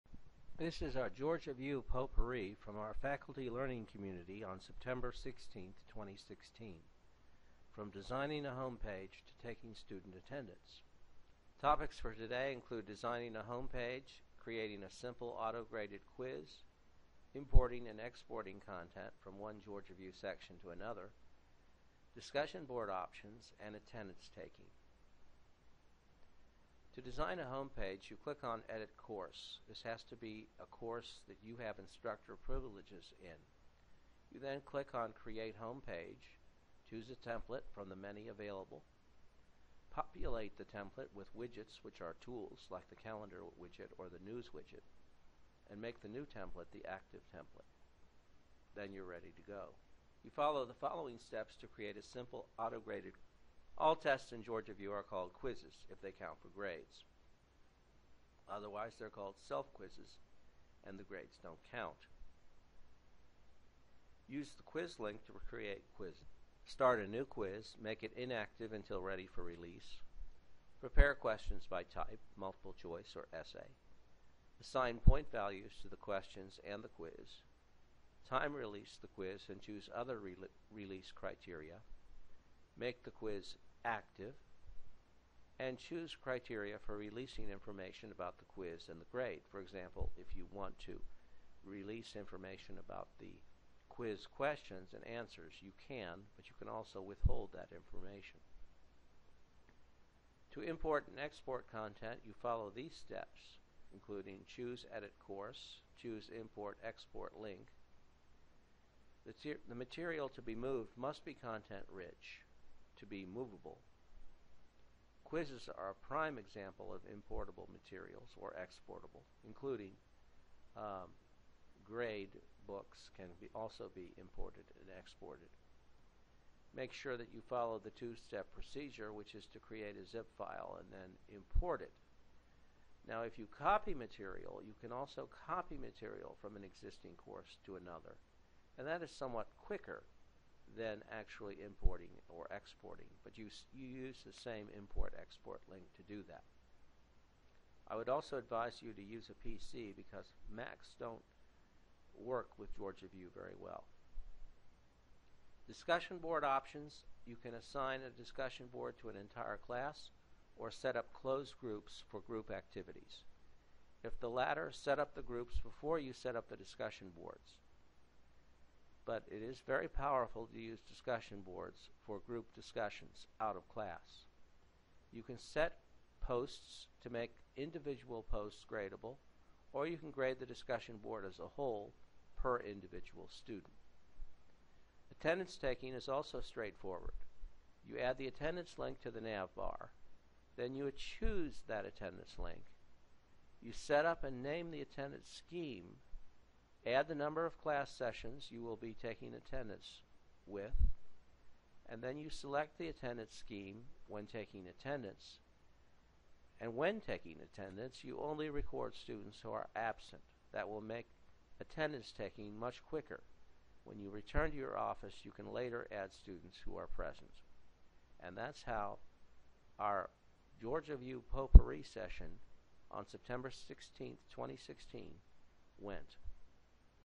GeorgiaVIEW Potpourri
A Faculty Learning Community session at South Georgia State College, on Friday, September 16, 2016.